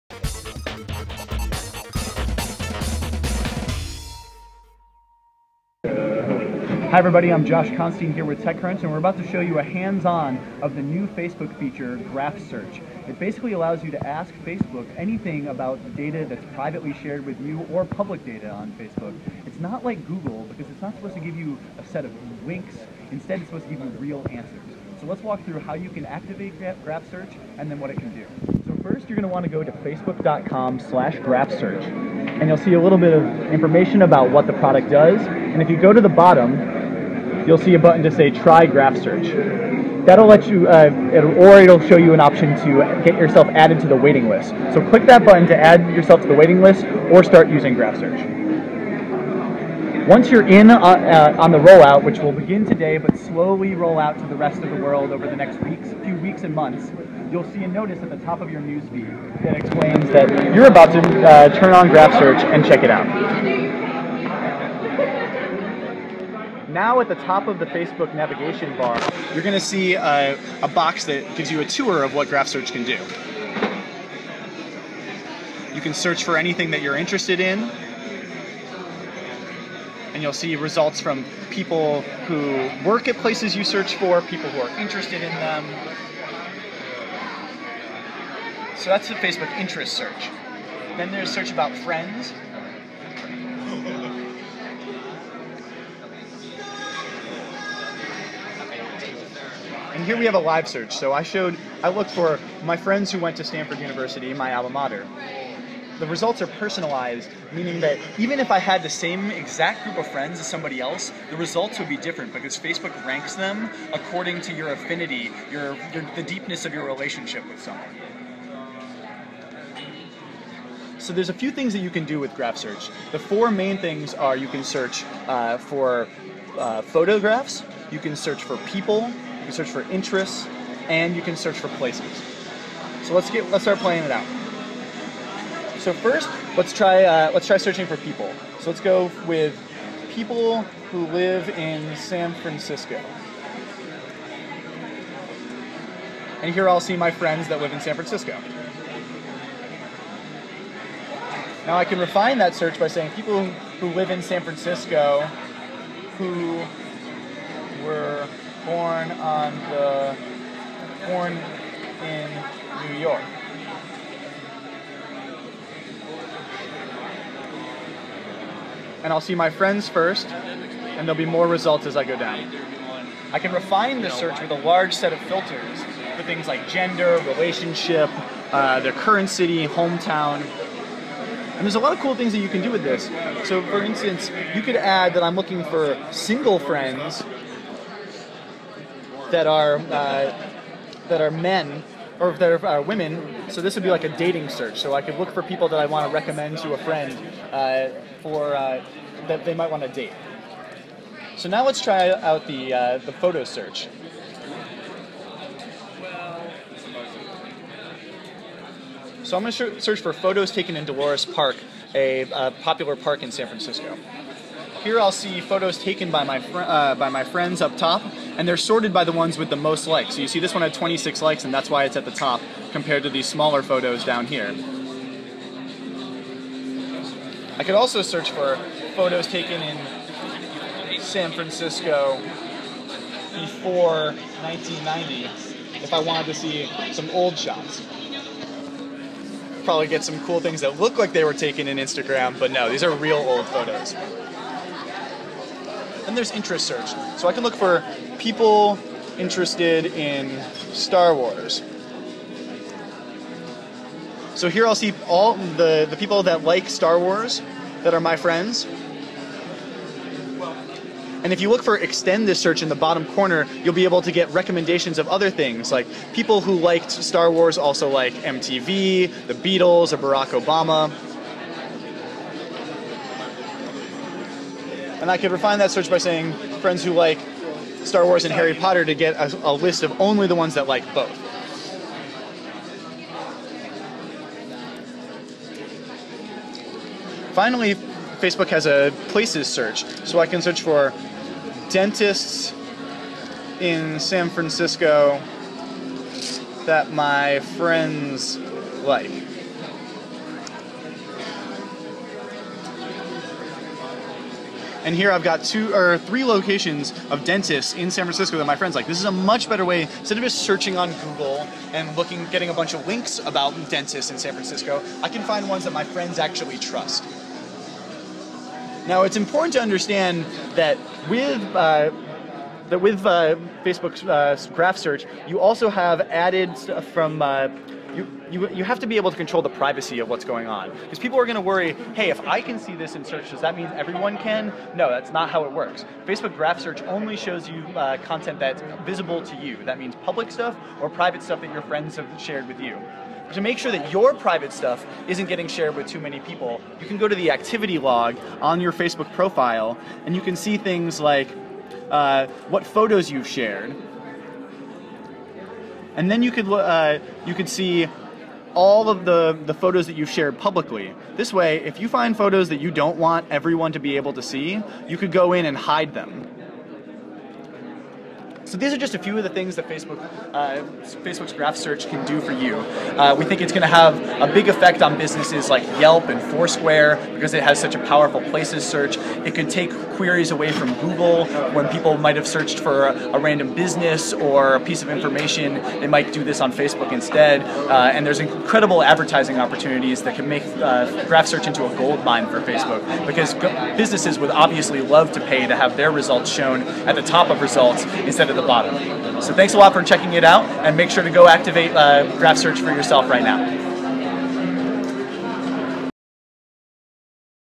Facebook Graph Search Demo
Josh Constine takes a look at the new Facebook Graph. Graph Search is Facebook's new search tool that allows you to type in a question or interests and the engine uses your networks data to provide you with an answer, not just links.